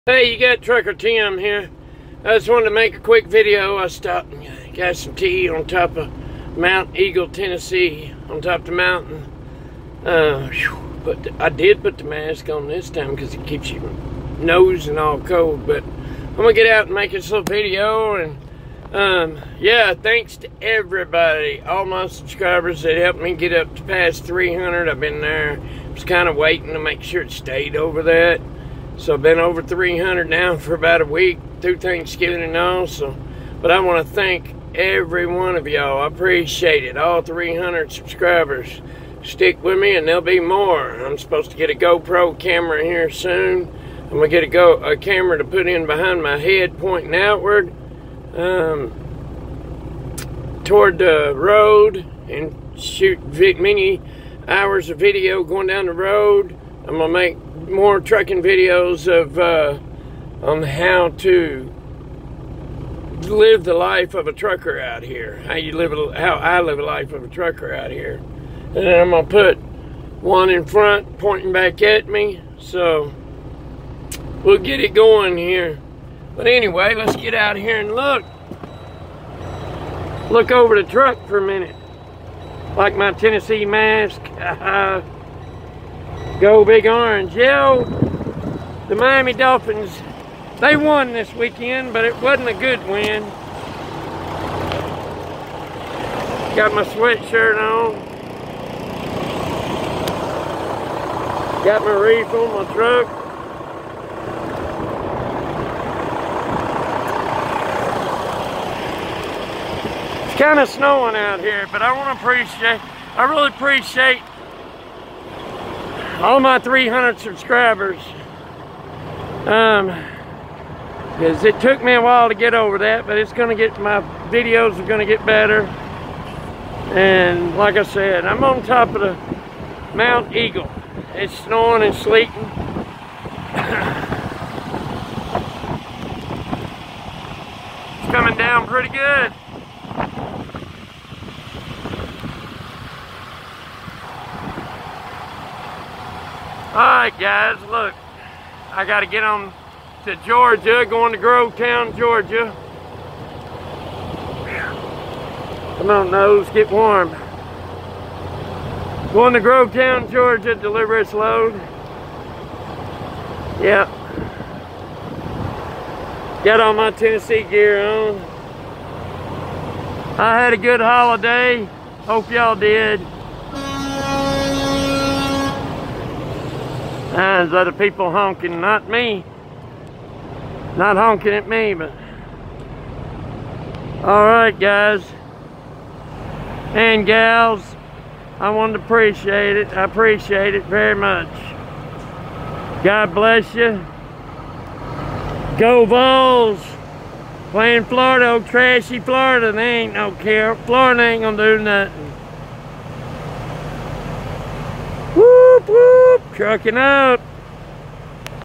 THANKS TO ALL MY SUBSCRIBERS AND ON TOP OF MONTEAGLE TN SNOWING
THANKS TO ALL THAT HELP ME GET OVER 300 SUBS IT MEANS ALOT

MONTEAGLE,TN